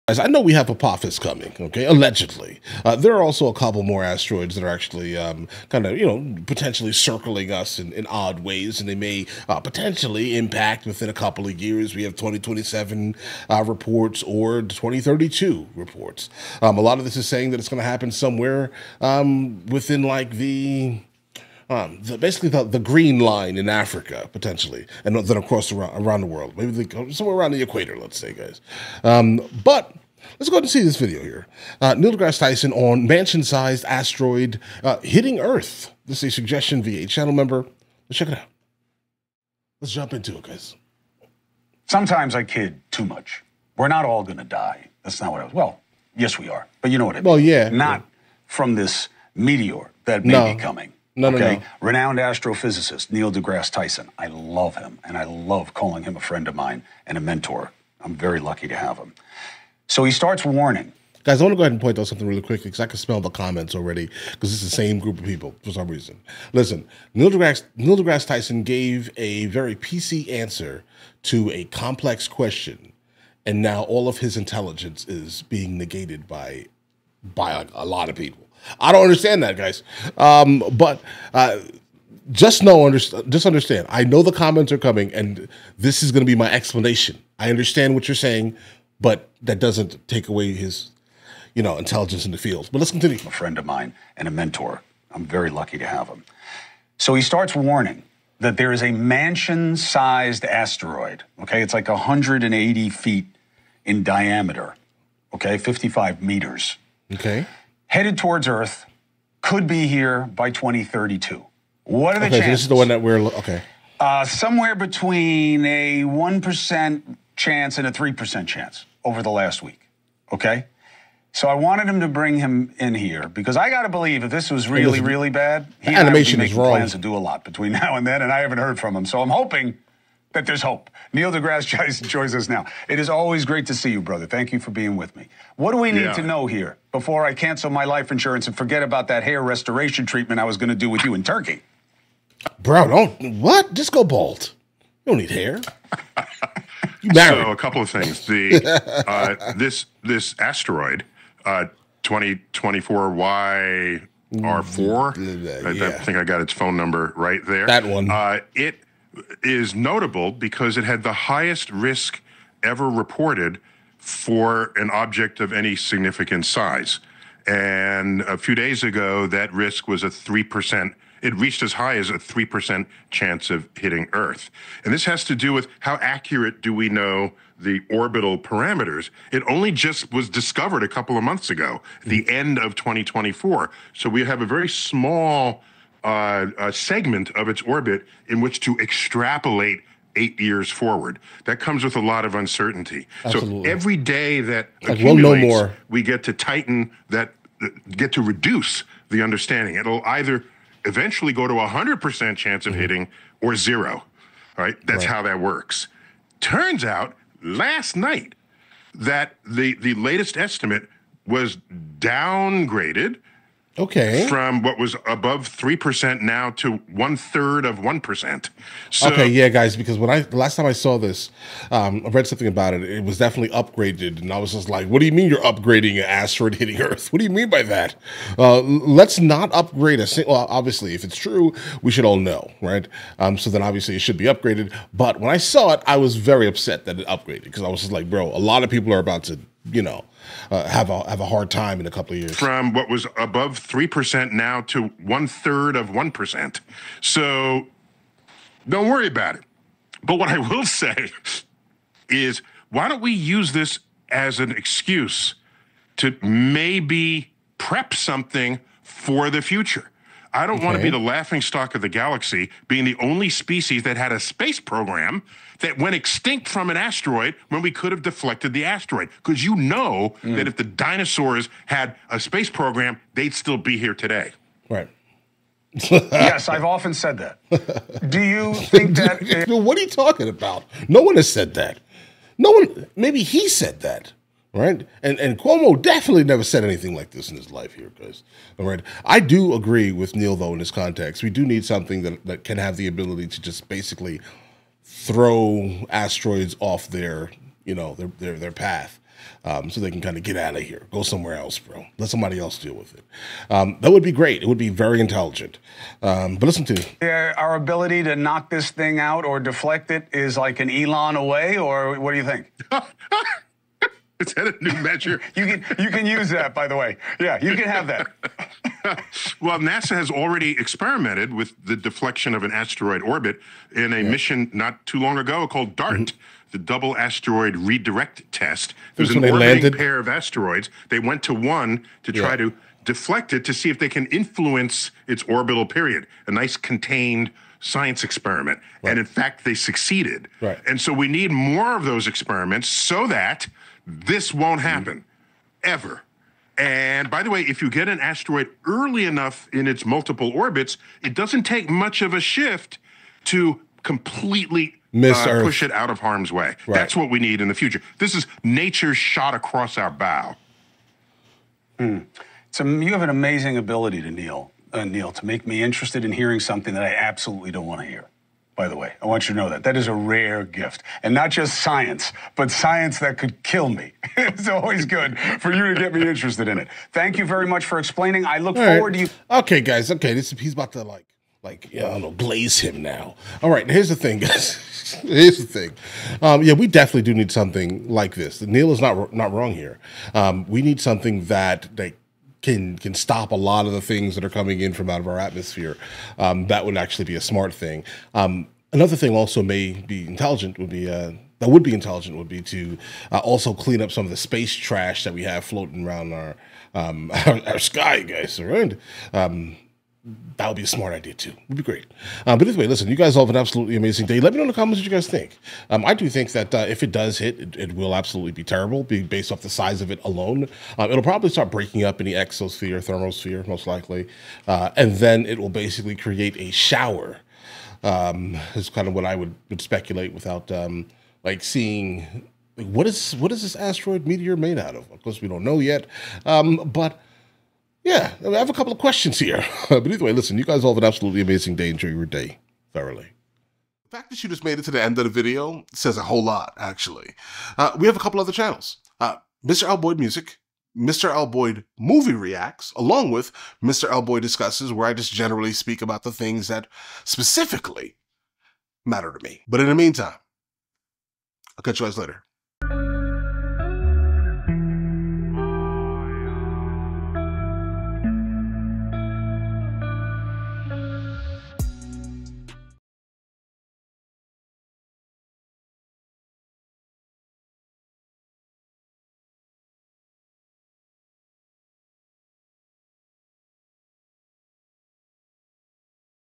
Mansion-Sized Asteroid MAY BE Headed Straight for Earth ..
The odds keep growing that an asteroid big enough to wipe out a city will collide with Earth in seven years, according to NASA. Astrophysicist Neil deGrasse Tyson to discuss how to stop the asteroid from hitting earth.

** ROAD TO 1,000,000 SUBSCRIPTIONS ***